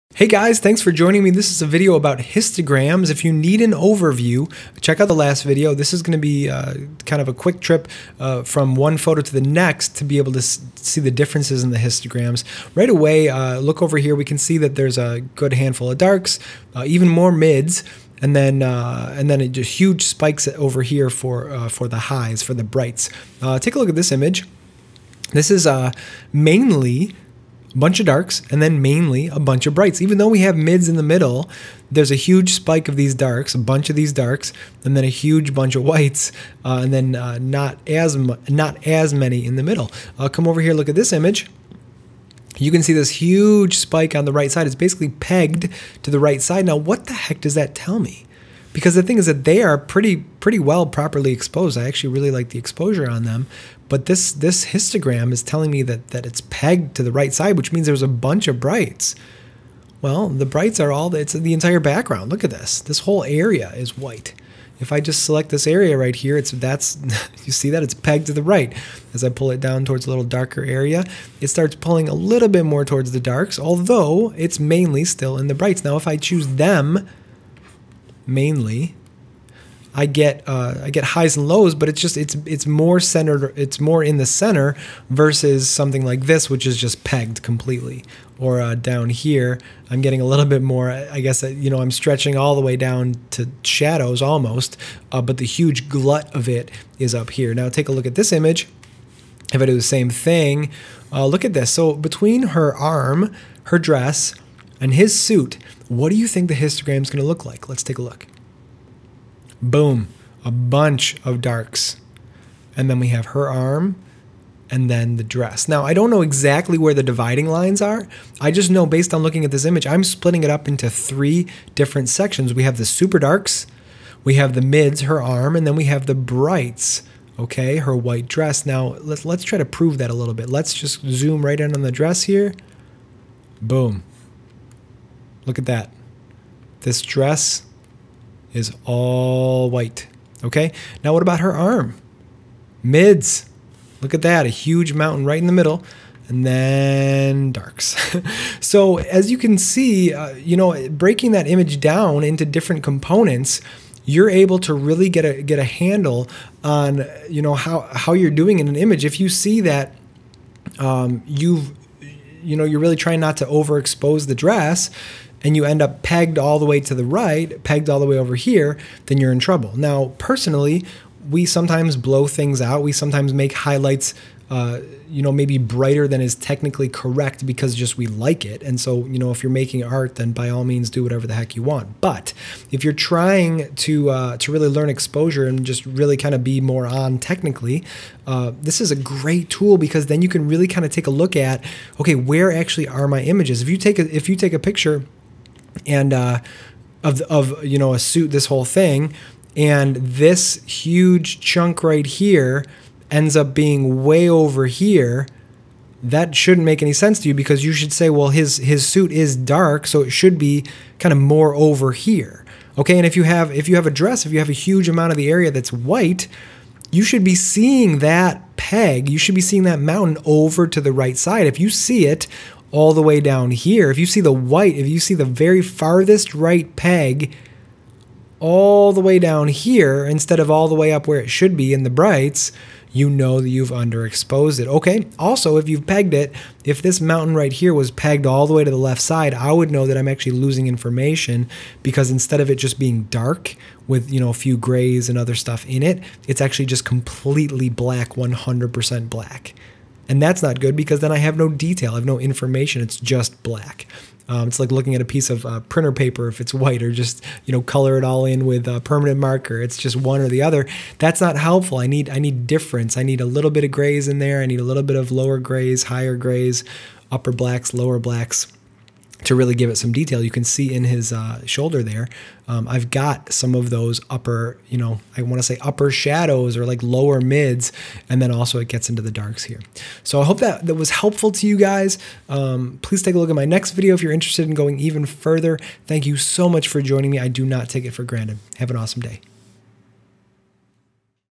What Is A Histogram? (Part 2): Differences between photos
Hey guys thanks for joining me! This is a video about Histograms (Part 2)

If you need an overview, check out the last video. This is going to be a quick trip from one photo to the next to be able to see the differences in the histograms.

Right away! Look over here and we can see that there's a good handful of darks, even MORE mids, and then a huge spike over here for the highlights/brights.

Take a look at this image -- this is mainly a bunch of darks and then mainly a bunch of brights. Even though we have mids in the middle, there's a huge spike of these darks, and then a huge bunch of whites and not as many in the middle.

Come over here and look at this image: you can see this huge spike on the right side, it's basically pegged to the right side -- now what the heck does that tell me?

The thing is that the couple in the photo is pretty well properly exposed, but this histogram is telling me that it's pegged to the right side which means there's a bunch of brights!

Well, the brights are high because of the huge area in the background. If I just select this area here, you can see it's pegged to the right. When I pull the box down to the darker area, the histogram starts pulling more down towards the darks although it's mainly still in the brights.

Now if I choose the couple mainly, I gets highs and lows but the histogram is more in the center, versus something like this which is pegged completely.

Or down here, I'm getting a little bit more, stretching all the way down to shadows, but the HUGE glut of it is in the highlights.

Now take a look at this image -- look at this, between her arm, her dress, and his suit, what do you think the histogram is going to look like?

BOOM. A bunch of darks. And then we have her arm and then the dress.

Now I don't know exactly where the dividing lines are, I just know based on this image, I'm splitting it up into three different sections. We have the super darks, the mids (her arm), and then the brights (her white dress).

Let's try to prove that a little bit -- let's zoom in on the dress and BOOM. Look at that. This dress is ALL white. Now what about her arm? MIDS. A huge mountain right in the  middle of the histogram. And then, darks. So as you can see, you know, breaking that image down into different components, you're able to really get a handle on how you're doing in an image.

If you see that you're really trying not to overexpose the dress and you end up pegged all the way to the right, then you're in trouble.

Now personally, we sometimes blow things out (make highlights brighter than is technically correct because we like it). If you're making art, do whatever the heck you want!

But if you're trying to really learn exposure and really be more ON technically, this is a great tool because you can take a look at where ACTUALLY the exposure is.

If you take a photo of a suit and this huge chunk right here ends up being way over here, that shouldn't make any sense to you, because his suit is dark so the histogram should show more of a mountain on the left side, near the darks.

If you have a huge amount of the area that is white, you should be seeing that mountain over on the right side. If you see the FARTHEST RIGHT mountain all the way down towards the shadows, instead of all the way up near where it should be in the brights, you know that you've underexposed.

If there is a mountain pegged all the way to the left side, I would know that I'm actually losing information because instead of it just being dark with a few grays and other stuff in it, it's just COMPLETELY black, 100% black. And that's not good because then I don't have any detail, it's just black. Like looking at a piece of printer paper (all white) or colored all in with permanent marker (all black), that's not helpful. I need the DIFFERENCE of those low tones, I need grays and upper and lower blacks to get that detail in the suit.

You can see in his shoulder, you can see that I've got upper shadows or lower mids and then also it gets into the darks here.

Hope that was helpful!